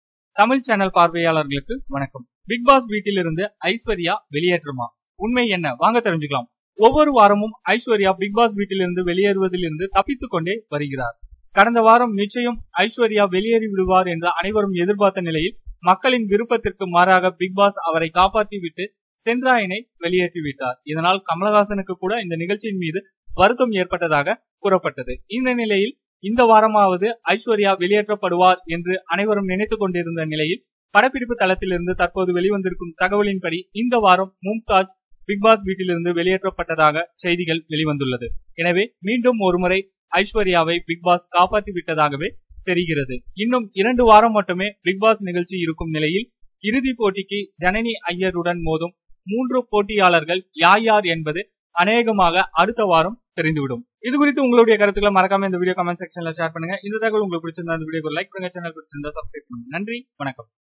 பிக்பாஸ் வீட்டில் இருந்து ஐஸ்வர்யா வெளியேற்றமா? பிக்பாஸ் வீட்டில் திடீர் திருப்பம்!
பிக்பாஸ் வீட்டில் இருந்து ஐஸ்வர்யா வெளியேற்றமா?